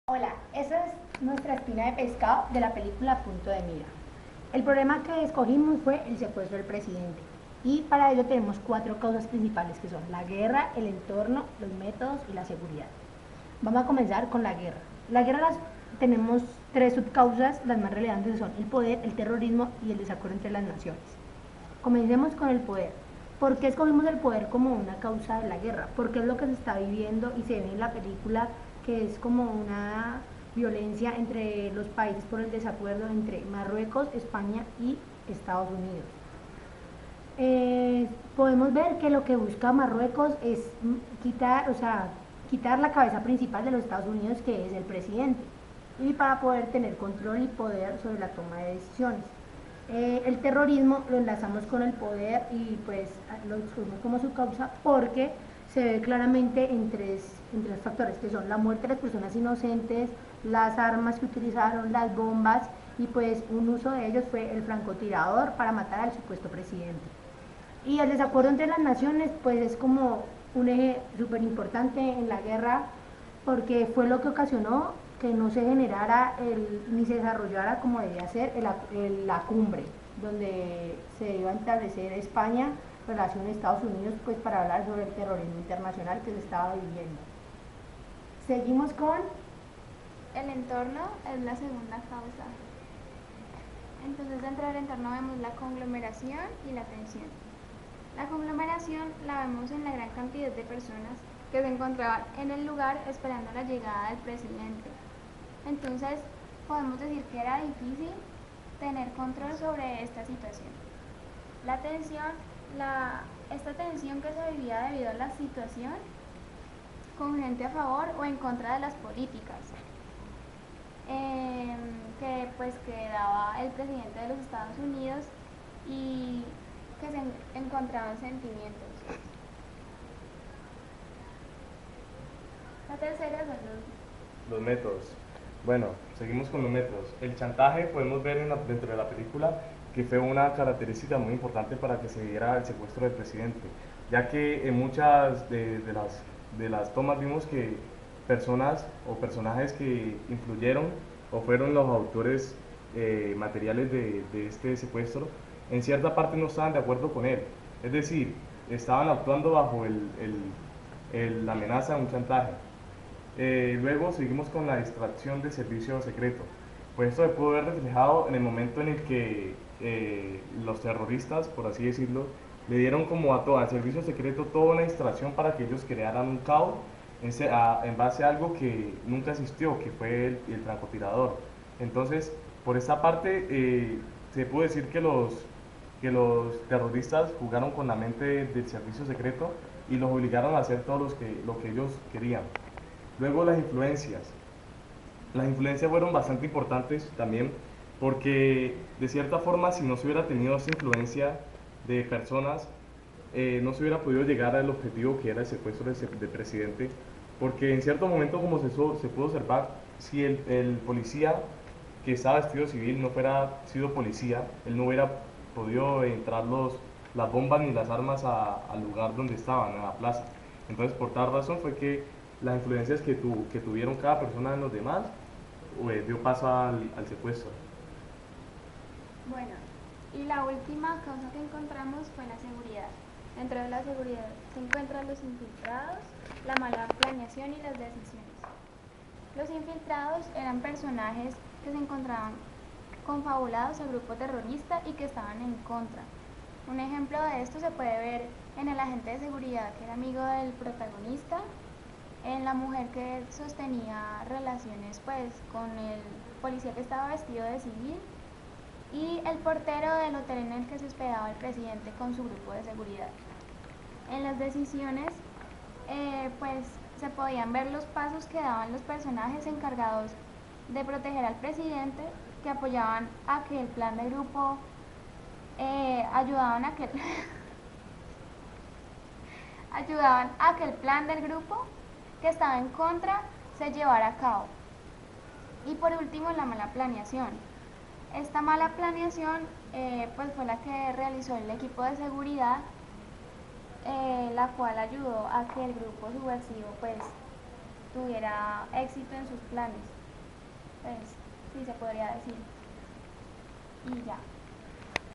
Espina de Pescado 1
En este video damos explicación de la primera Espina de Pescado sobre la película "En el Punto de Mira" . El problema a tratar es: El secuestro del Presidente de EE.UU (hipotético en la historia).
Integrantes: Robert, Ma. Alejandra P, Lindsay